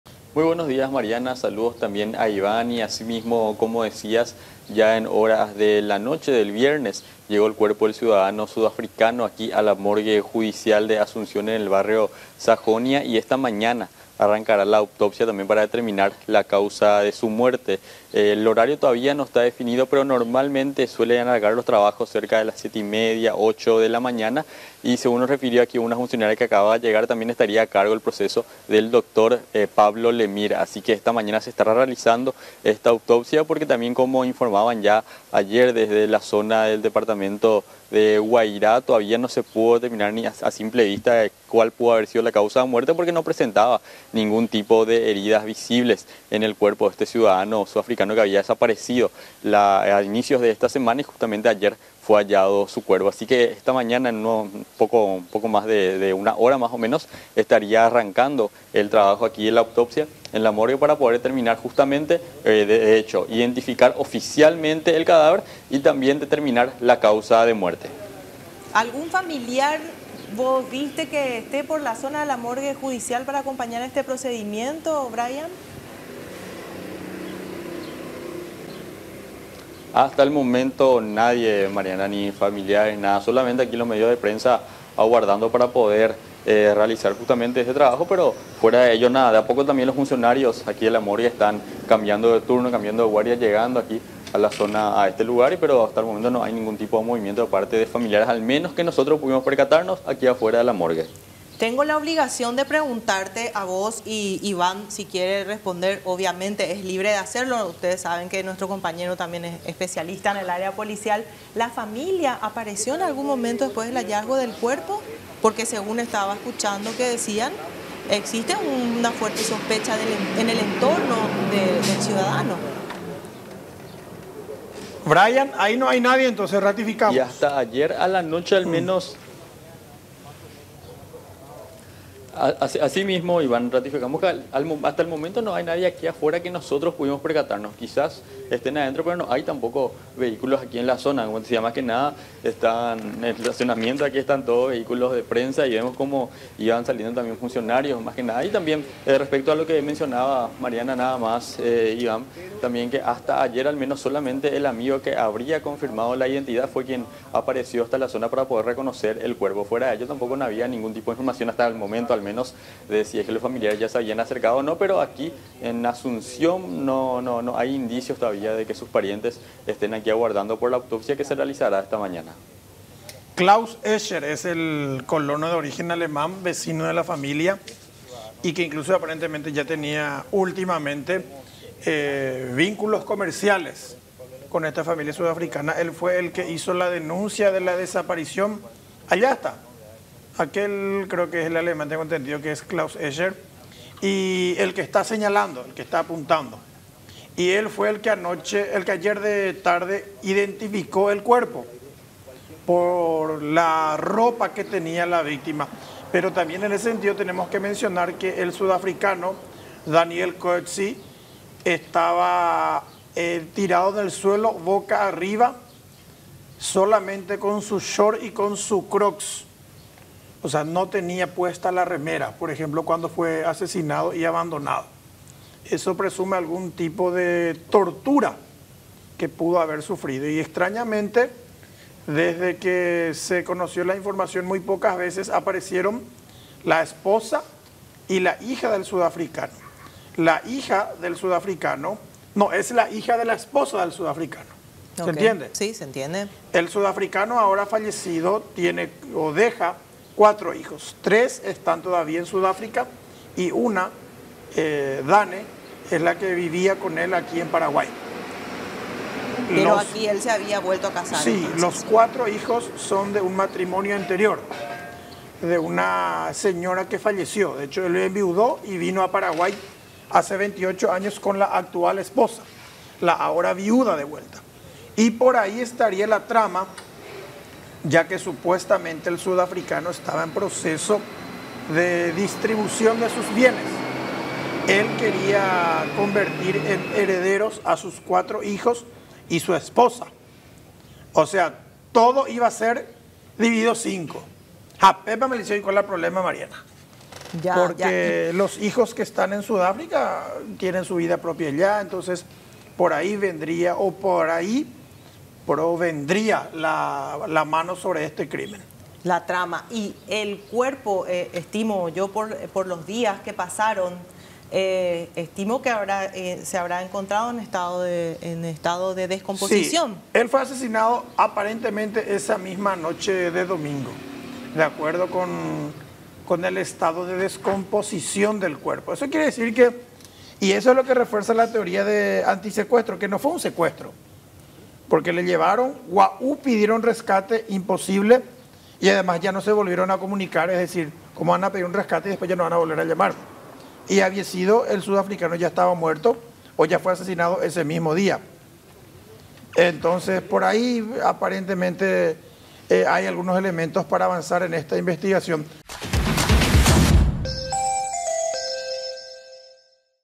Crimen de ciudadano sudafricano
El sudafricano Daniel Christiaan Bruewer Coetzee, de 75 años, fue encontrado muerto en medio de plantaciones de caña de azúcar y yerba mate, a cinco kilómetros de su establecimiento del distrito de Independencia de donde fue raptado el domingo último. Esta mañana estarían realizando la autopsia del cuerpo.